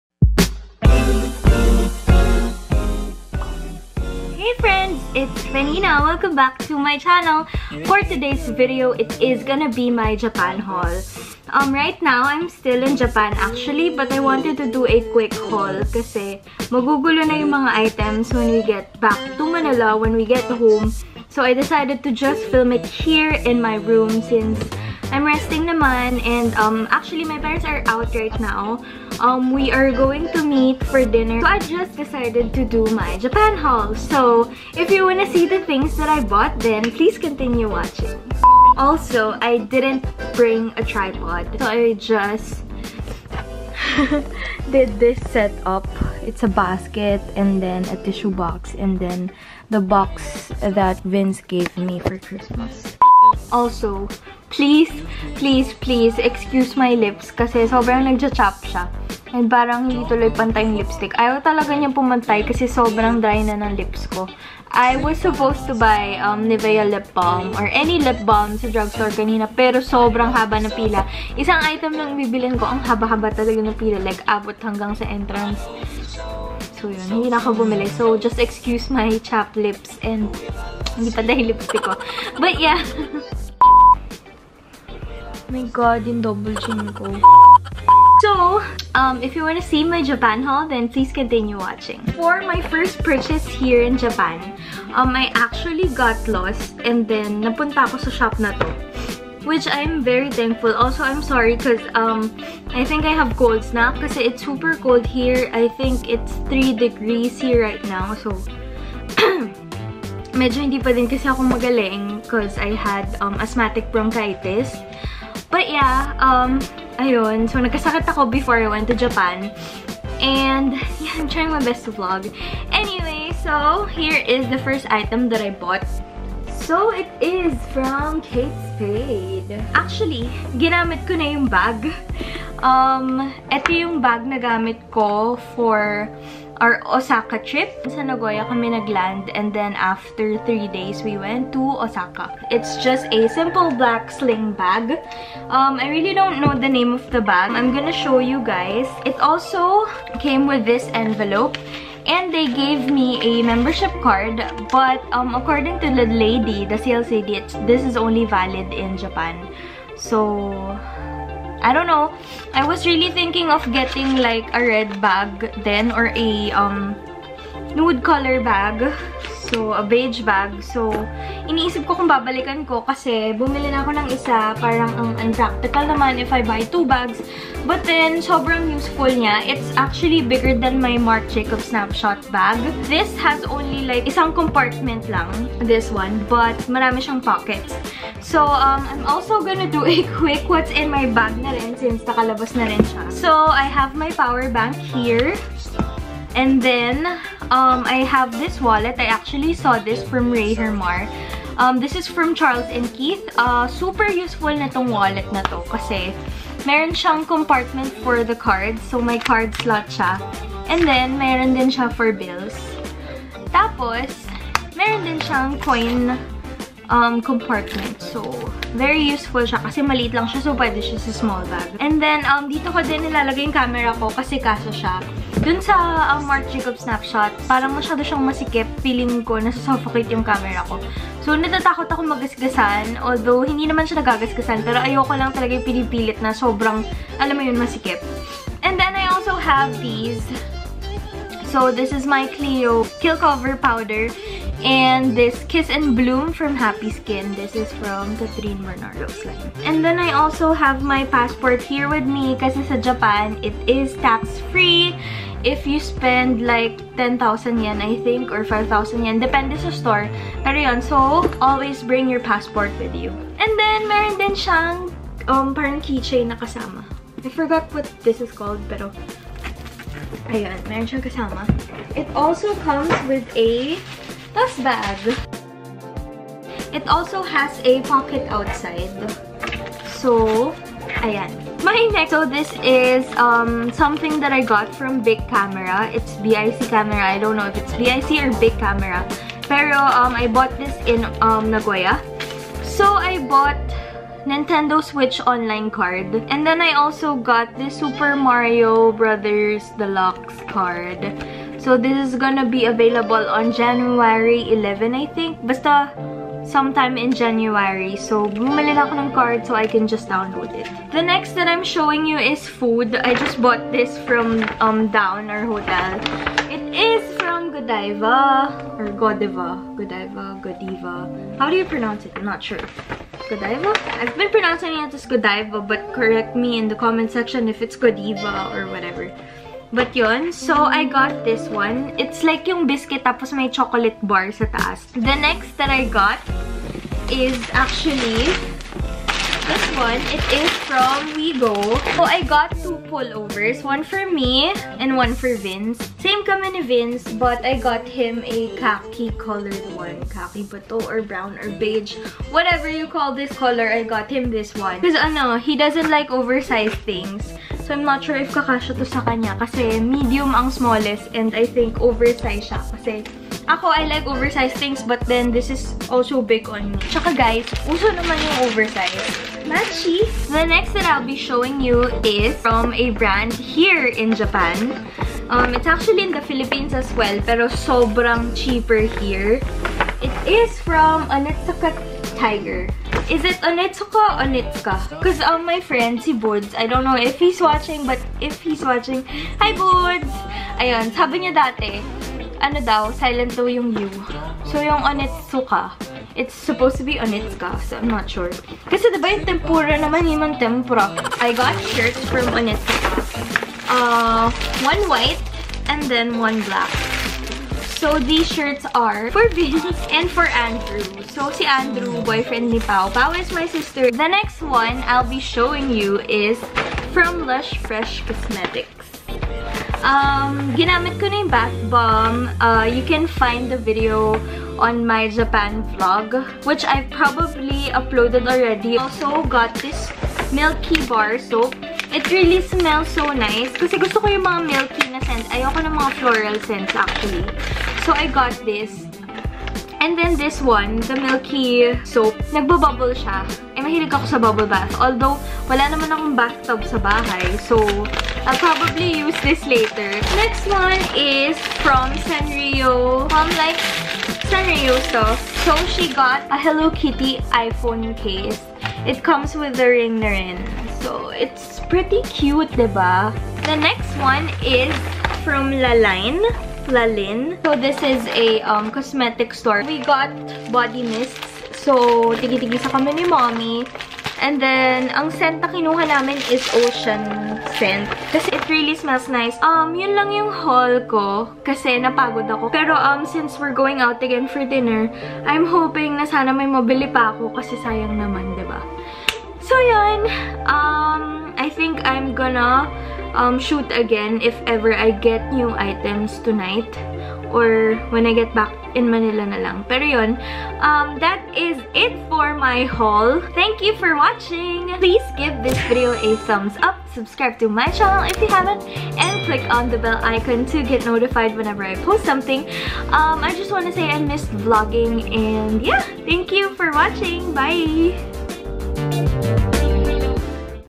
My Japan Haul! (Onitsuka, Kate Spade, Fila, etc!) | Lifestyle vlog | Fenina Tolentino
Hey, friends!

I am on fire this week!!! Yesss! It is the first week of the year 2019! And I feel so productive, bwaha!

Part 1, cause I wasn't able to shoot my last minute shopping yet (mostly jbeauty & food) -- PLEASE DO WATCH OUT FOR IT!